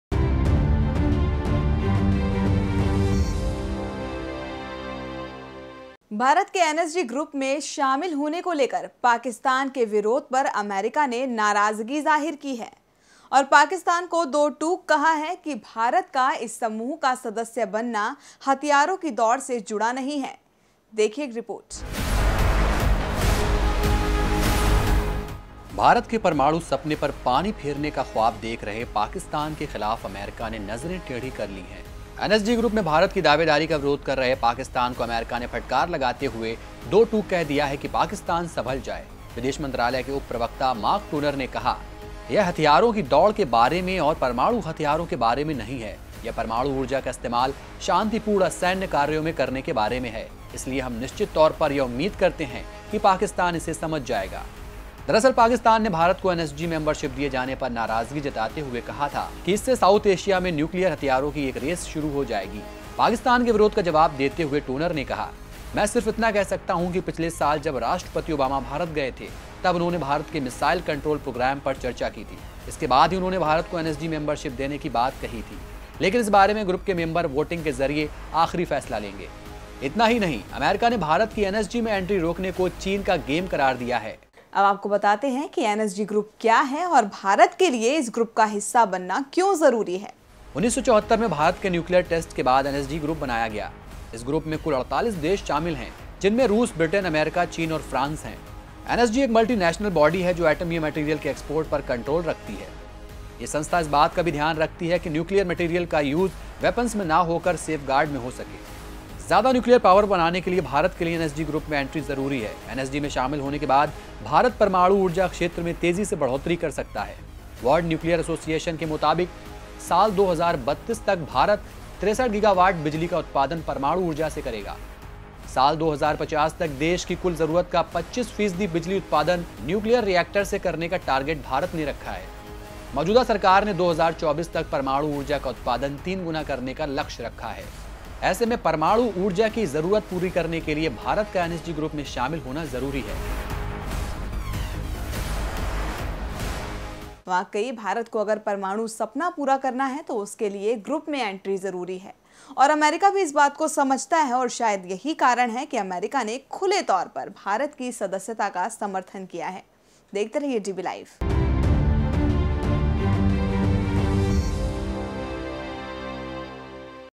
DB LIVE | 28 MAY 2016| US says, India's NSG membership is not about arms race,
भारत के एनएसजी ग्रुप में शामिल होने को लेकर पाकिस्तान के विरोध पर अमेरीका ने नाराजगी जाहिर की है...और पाकिस्तान को दो टूक कहा है कि भारत का इस समूह का सदस्य बनना... हथियारों की दौड़ से जुड़ा नहीं है